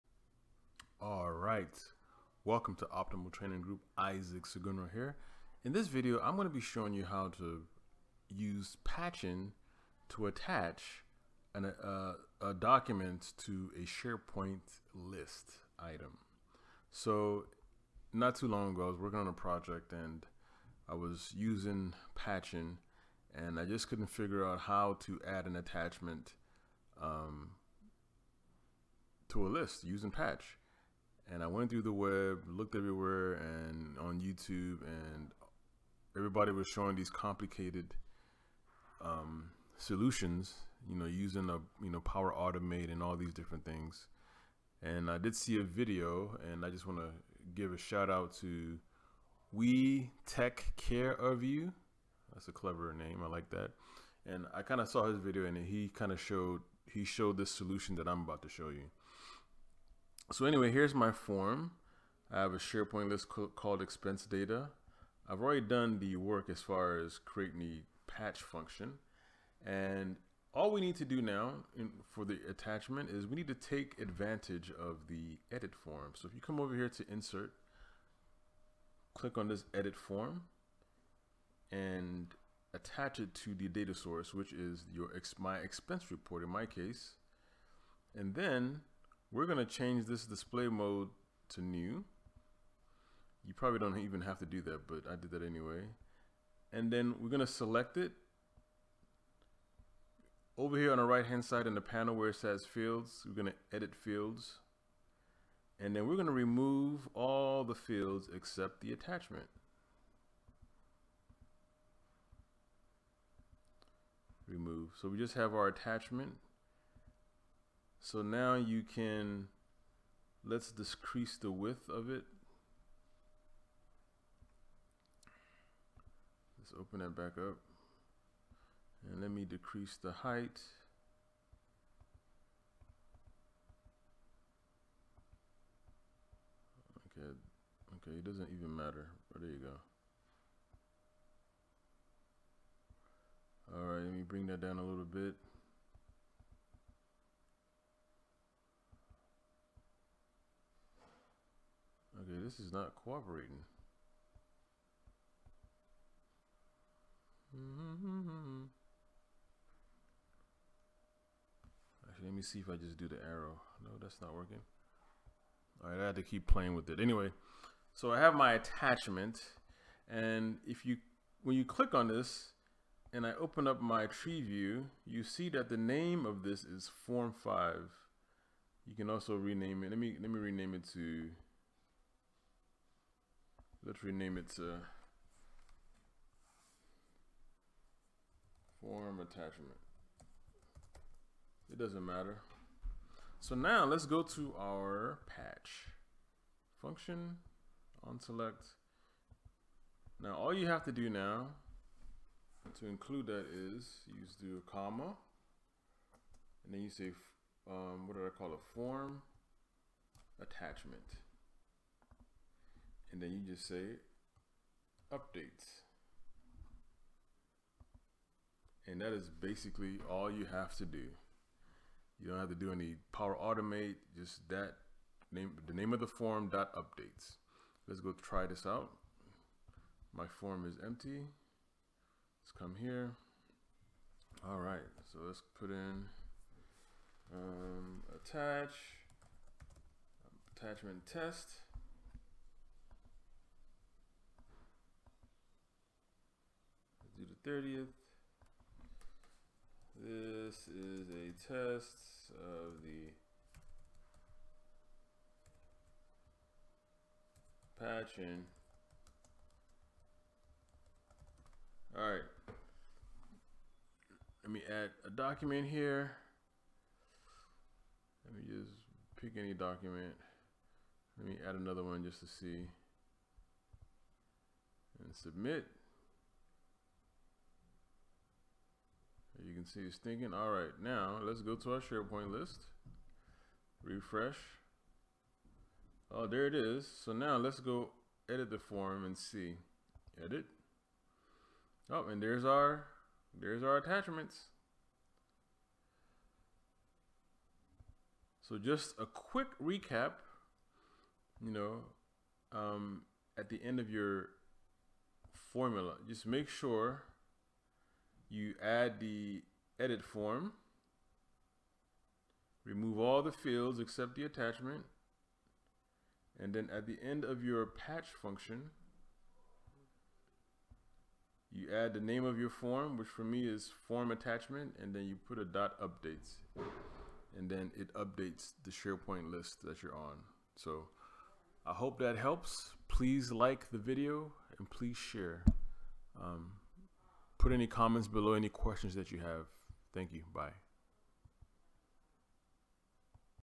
Learn a simple way to attach documents to a SharePoint list with Power Apps using Patch()
🚀 Dive into the world of Power Apps and SharePoint integration with this comprehensive tutorial! 🌟 In this video, I'll walk you through a step-by-step guide on how to use the Patch() function to seamlessly attach documents to a SharePoint list. Whether you're building custom forms or looking for a more efficient way to handle file attachments, this video is your go-to resource.

🔍 We'll cover:

How to set up the 'Edit Form' control in Power Apps for attachment handling.
Crafting and understanding the Patch() function to not just update list fields, but to also attach documents effectively.
Insights on leveraging Power Apps’ inherent capabilities for a streamlined workflow.
📌 Perfect for Power Apps enthusiasts, SharePoint administrators, and anyone looking to enhance their digital workspace with efficient data handling techniques. From beginners to advanced users, this tutorial offers valuable tips and tricks to make your Power Apps and SharePoint experience more productive.

Drop your questions and feedback in the comments below – let's make this a collaborative learning journey!